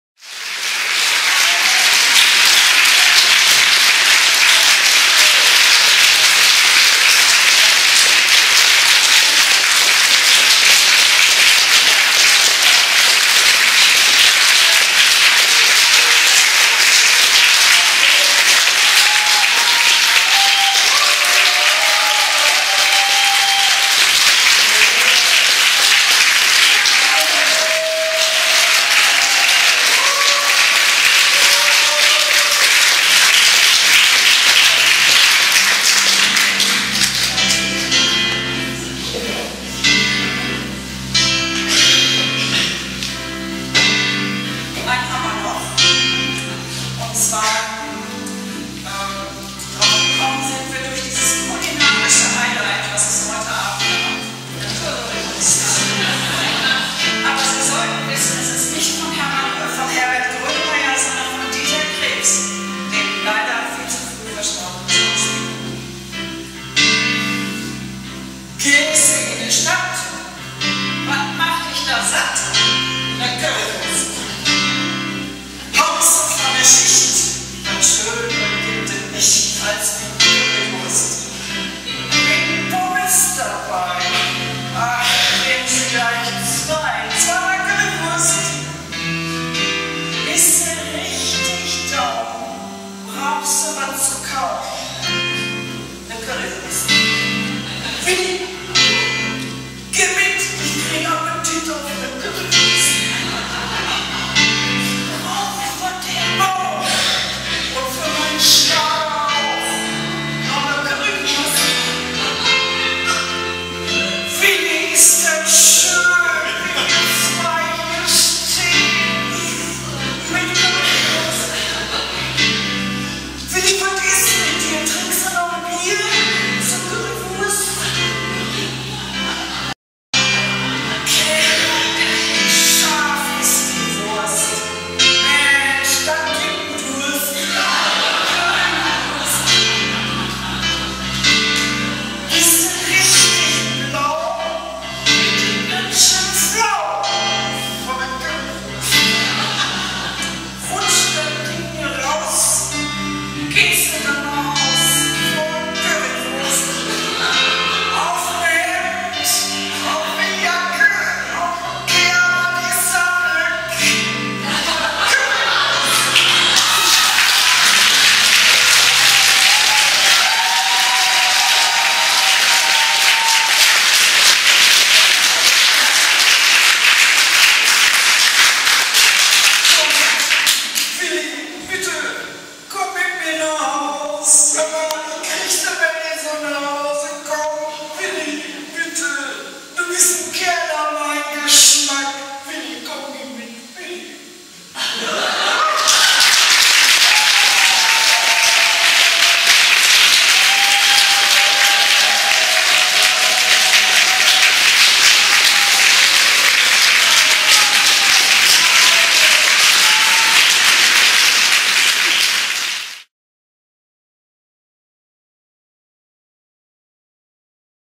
Currywurst von Horst-Herbert Krause, Poesie trifft Musik
Elisabeth Dieckhöfer liest Currywurst von Horst-Herbert Krause, Piano: Olaf Nuhnen, Zugabe des Auftrittes im September 2015 in Henrichenburg